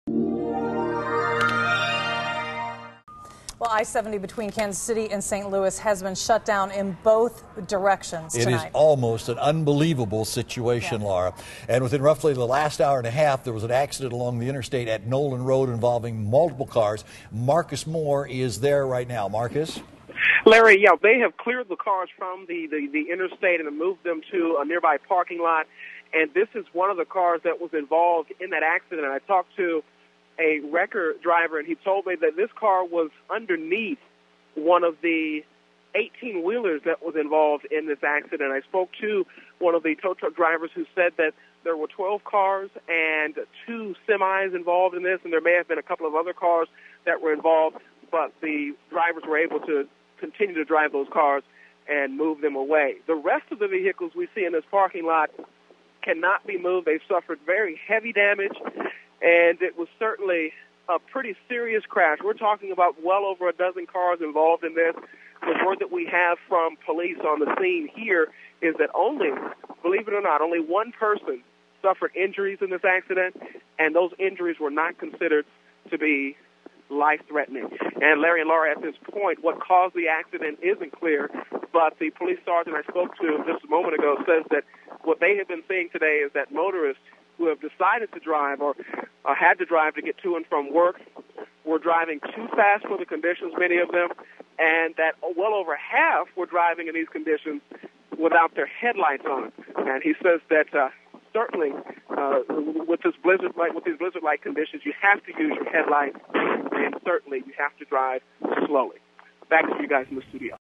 Crews Clear Huge Pileup Off I-70
A crash that involved as many as 15 cars on I-70 near Noland Road was cleared just before MoDOT closed the highway statewide. One person suffered an injury in the crash. KMBC 9's Marcus Moore reports.